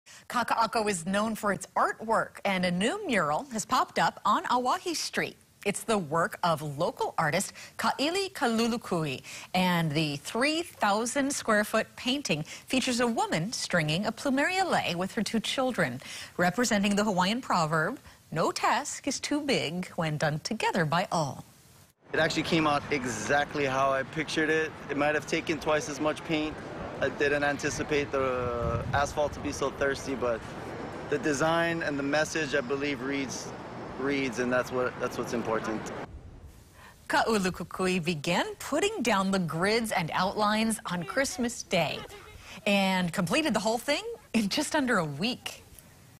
New Kakaako mural located at SALT at Our Kaka'ako
A new on-street mural is now completed at SALT at Our Kaka'ako.